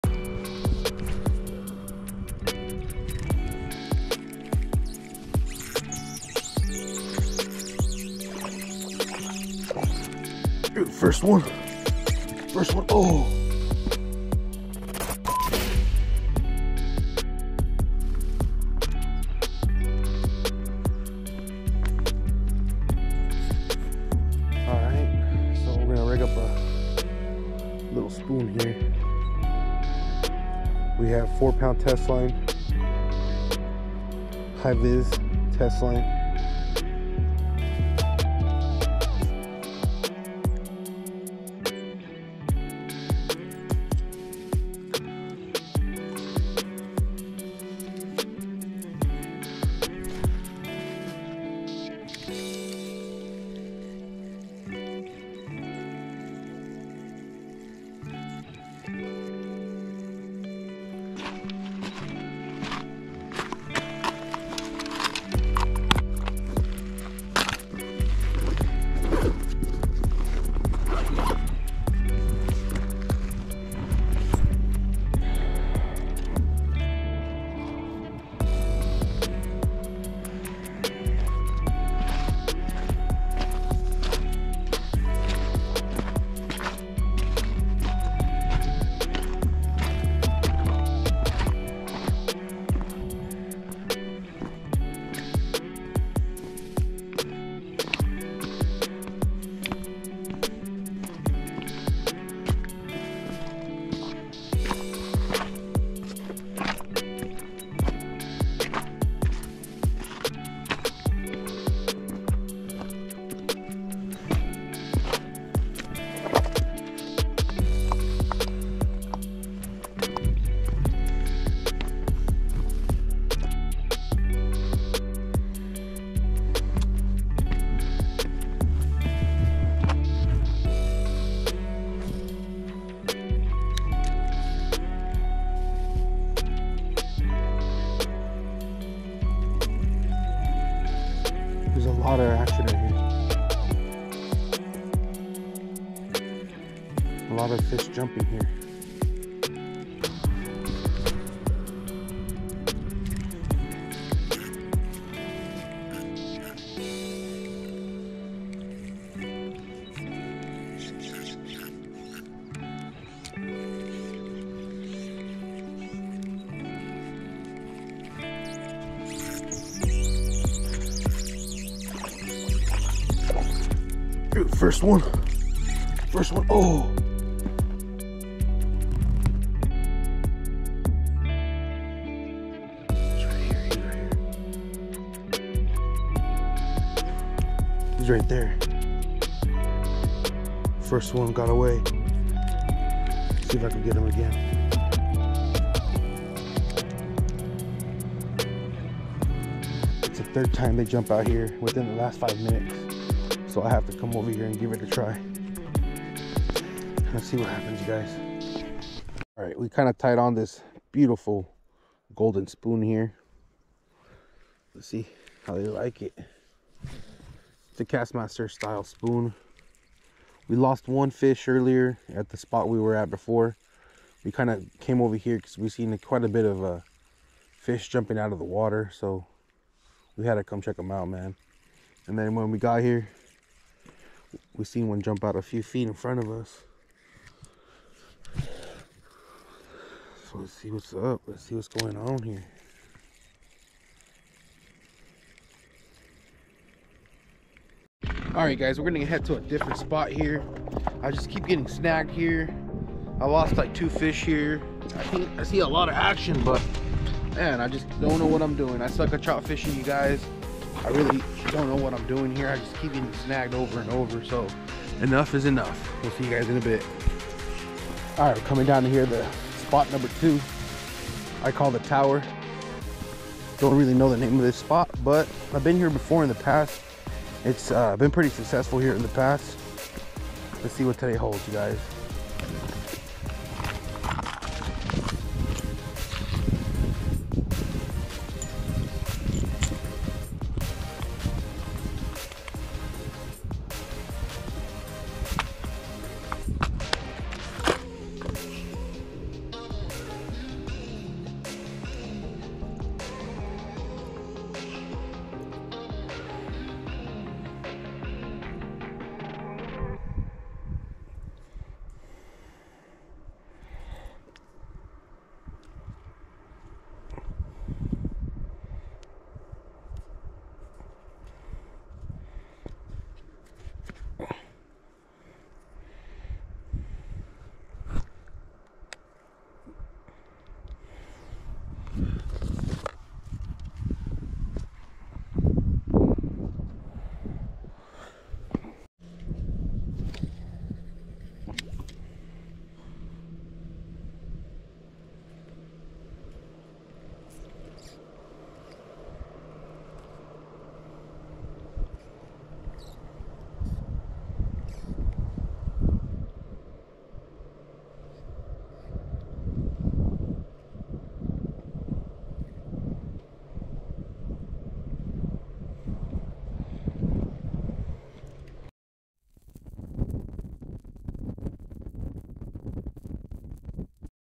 Trout fishing the Lower kings River (LKR)
Came out to the Lower kings River (LKR) and tried my luck . First spot was behind Winton park. I was able to hook onto some fish but I forgot my net and I tried flipping him to the bank. This was difficult as I was Fishing on top of some boulders!

second spot was a few miles west from Winton Park. I called it the Tower area.

Hopefully I have better luck next time!